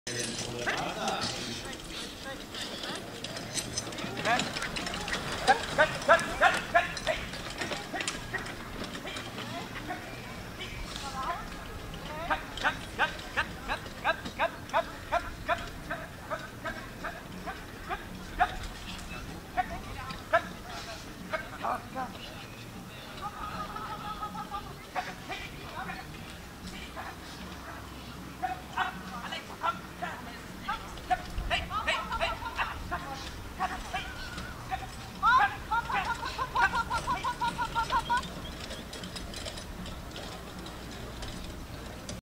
Fahrturnier Ostenfelde 18.8.2024
Ein kurzes Video einer Marathon-Fahr-Prüfung vom Fahrturnier in Ostenfelde
Münsterländer Mannschaftscup
Kreispokal
Kreismeisterschaften
Westfälische Jugendmeisterschaft
Bilder von verschiedenen Turniergelände bei Instagram:
Pferde.Flo